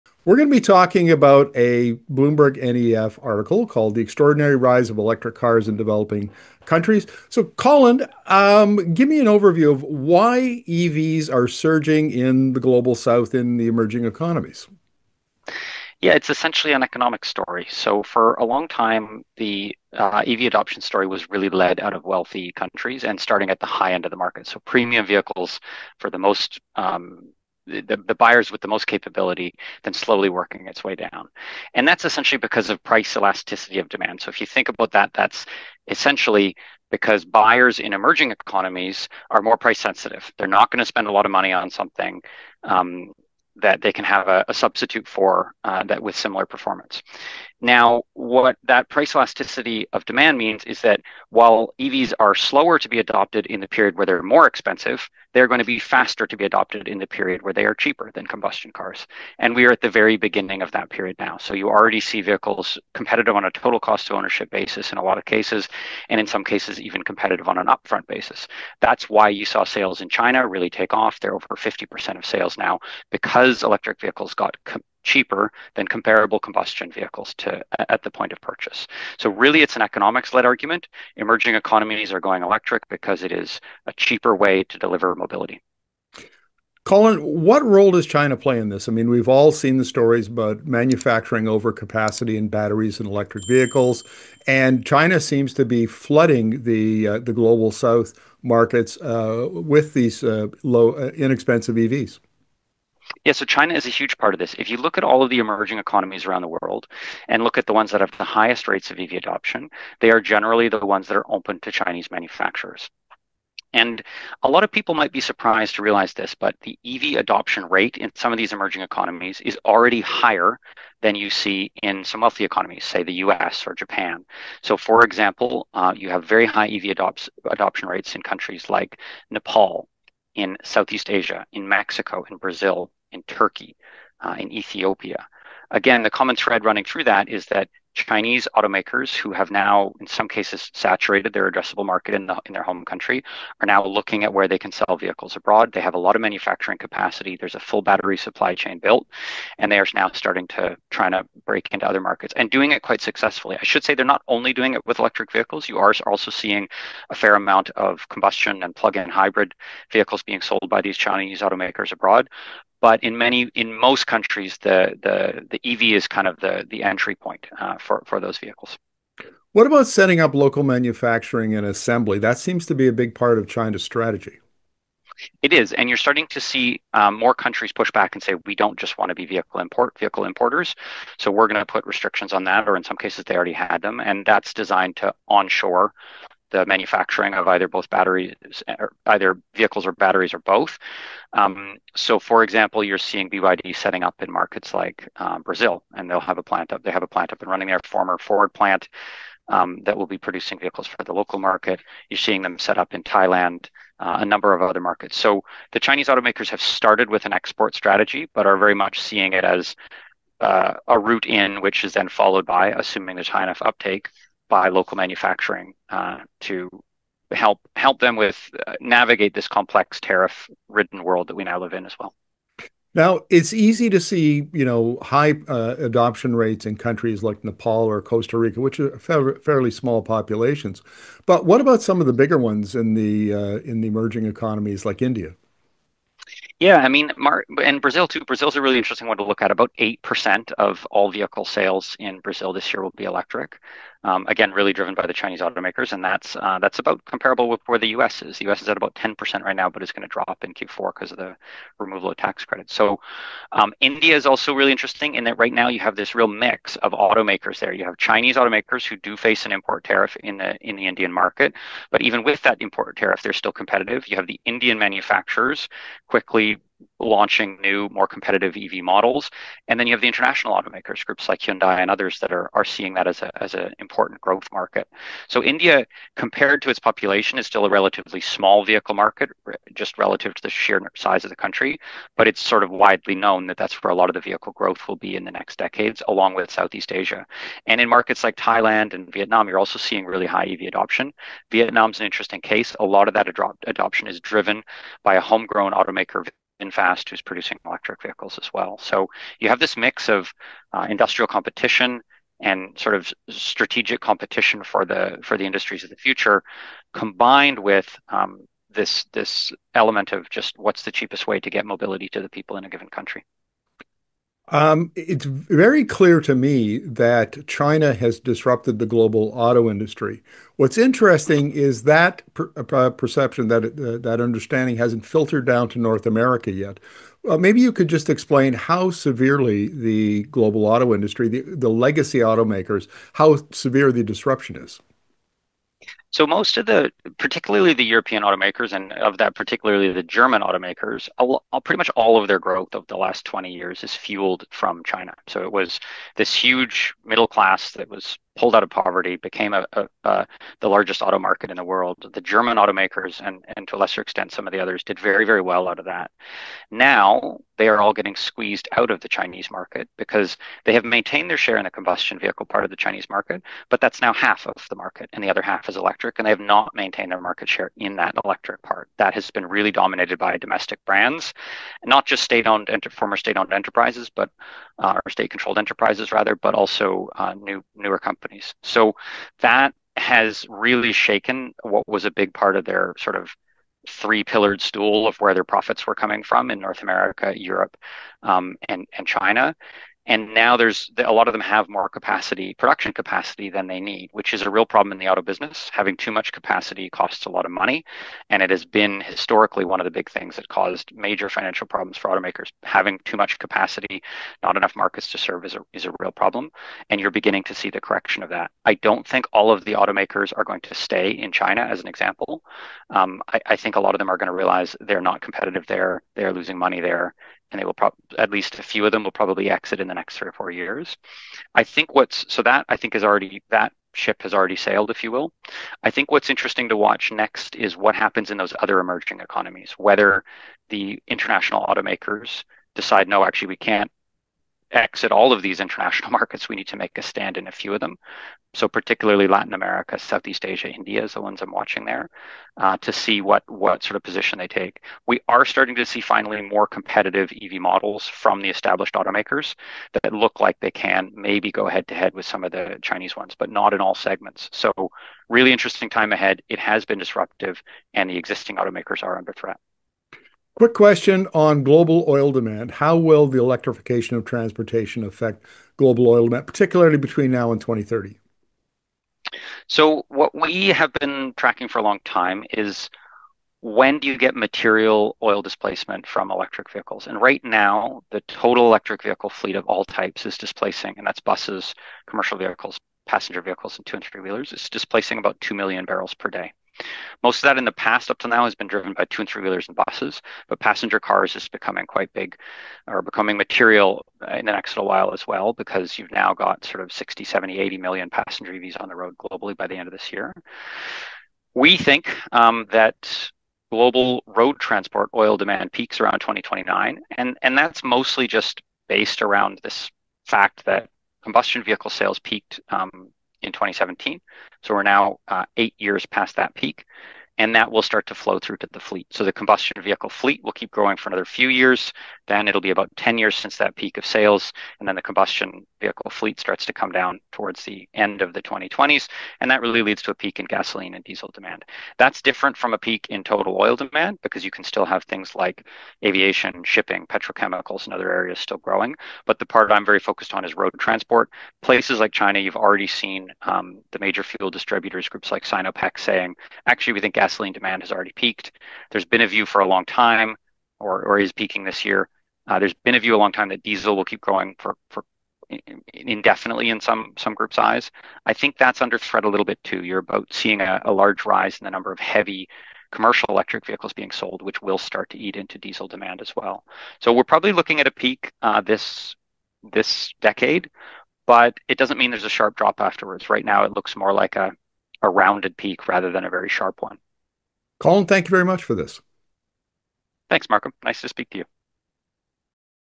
Why Emerging Economies Choose EVs Over Gasoline
North Americans are oblivious to the dramatic rise of EVs in Asia, Latin America, the Middle East, and even Africa. EVs are simply better than internal combustion engine (ICE) cars: lower cost, higher value, better performance. The rapid expansion is driven by China, not the West or legacy automakers.